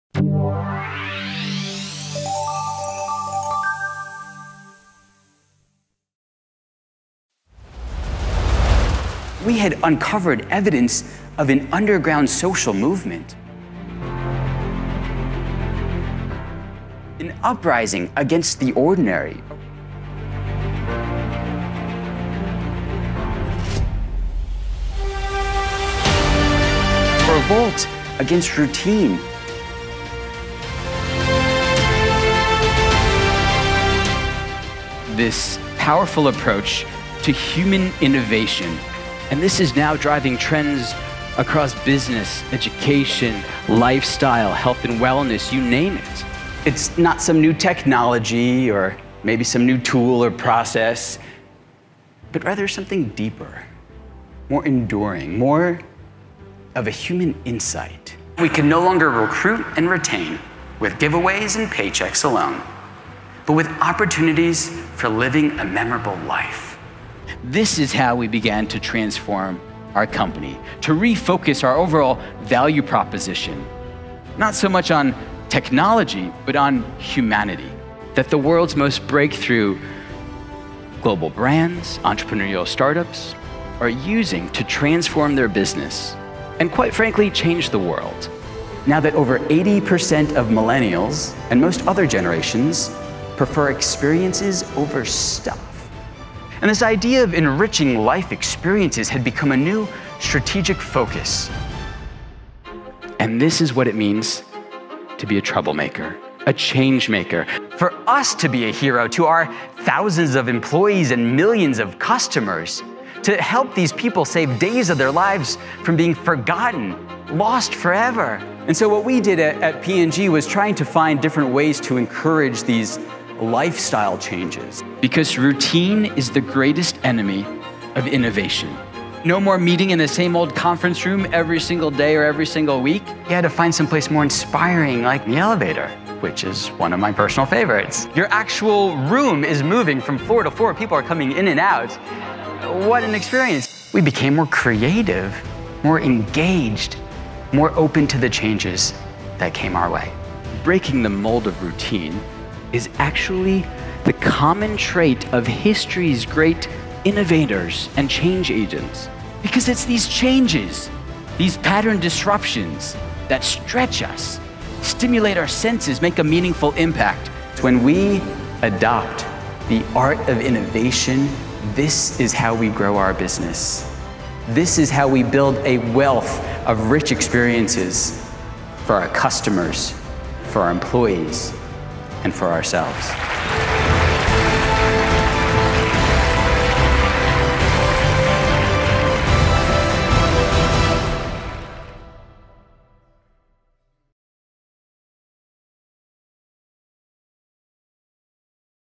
Speaking Reel - Dustin Garis
Dustin Garis is a world-renowned brand innovator, consistently voted “the #1 event speaker.” Throughout his disruptive career at some of the world’s most respected companies, such The Coca-Cola Company and Procter & Gamble, Garis has become known as “Chief Troublemaker” for his innovative approach to brand-building, customer experience, and change leadership.